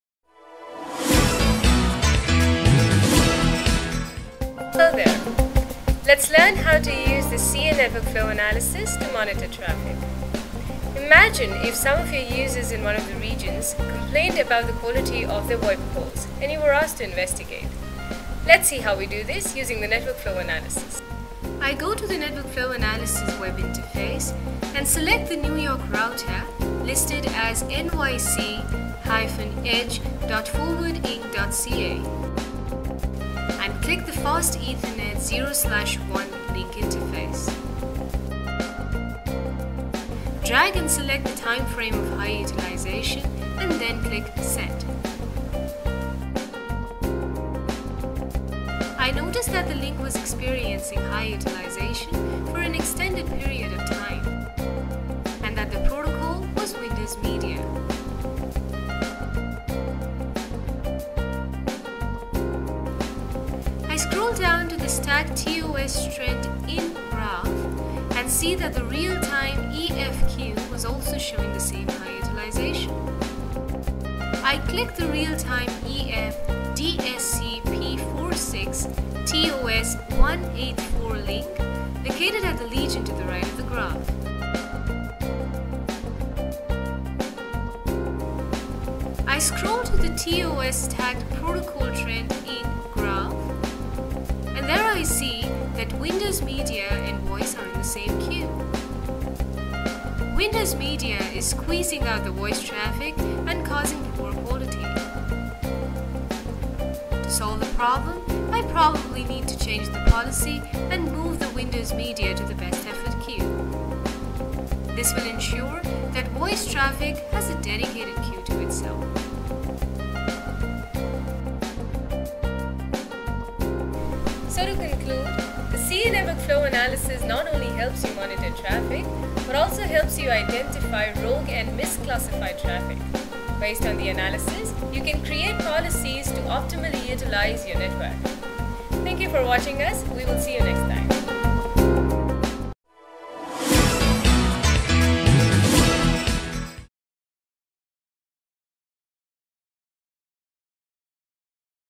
NFA - Applications with QoS Markings
In this video, you will learn how to investigate a complaint on the Quality of the VOIP calls.  Based on the analysis, you can create policies to optimally utilize your network.